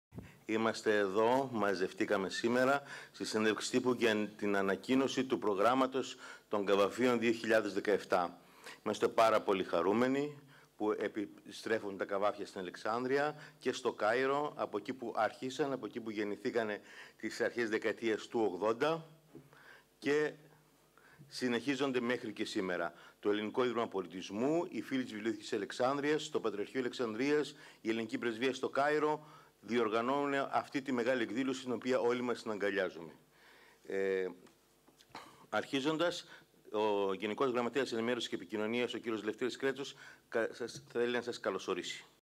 "Καβάφεια 2017", Συνέντευξη Τύπου, Γιώργος Φλωρεντής, Γενικός Γραματέας Υ.ΨΗ.Π.Τ.Ε.
Συνέντευξη Τύπου
Καβάφεια 2017 και εκδηλώσεις «Φίλων της Βιβλιοθήκης της Αλεξάνδρειας»
Κάιρο & Αλεξάνδρεια: 13 – 15 Οκτωβρίου 2017
Ο ΚΑΒΑΦΗΣ ΤΟΝ 21ο ΑΙΩΝΑ
Ο επίκαιρος χαρακτήρας του έργου του Κ. Π. Καβάφη

Την Τετάρτη 4 Οκτωβρίου 2017 και ώρα 12:30 πραγματοποιήθηκε Συνέντευξη τύπου στην Αίθουσα Εκδηλώσεων της Γενικής Γραμματείας Ενημέρωσης-Επικοινωνίας (Φραγκούδη 11 & Αλεξ. Πάντου, Καλλιθέα) όπου ανακοινώθηκε το πρόγραμμα του 14ου Λογοτεχνικού Συμποσίου "Καβάφεια 2017" και των εκδηλώσεων που διοργανώνονται στο πλαίσιο αυτό στην Αλεξάνδρεια και το Κάιρο. Το πρόγραμμα παρουσίασαν εκπρόσωποι του Ελληνικού  Ιδρύματος  Πολιτισμού, του Πατριαρχείου Αλεξανδρείας και των «Φίλων της Βιβλιοθήκης της Αλεξάνδρειας».